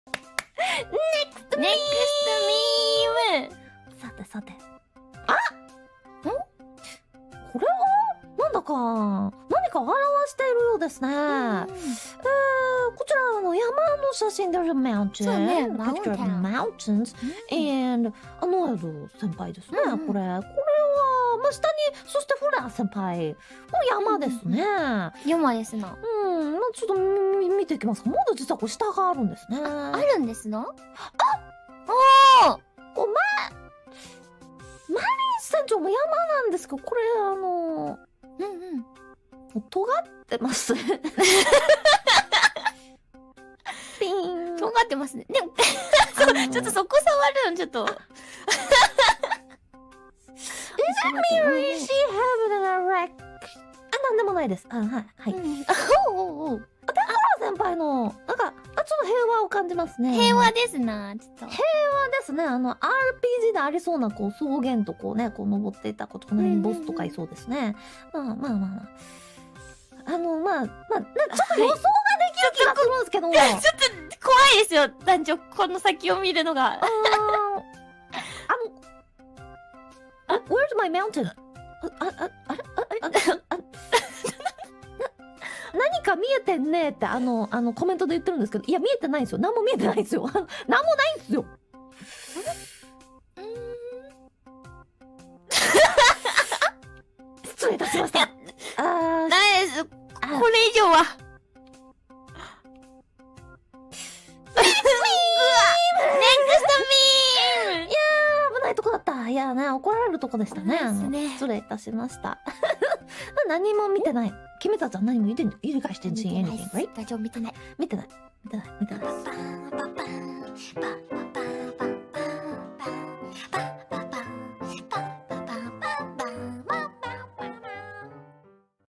[Hololive] Coco y Noel hablan de "Montañas" (SUB ESP)
La normal conversación de hablar sobre montañas entre Coco y Noel. Yo si escalaría esas 4 montañas si sabes a lo que me refiero.

Si tienes alguna sugerencia de algún clip que quieras ver traducido hazmelo saber en los comentarios.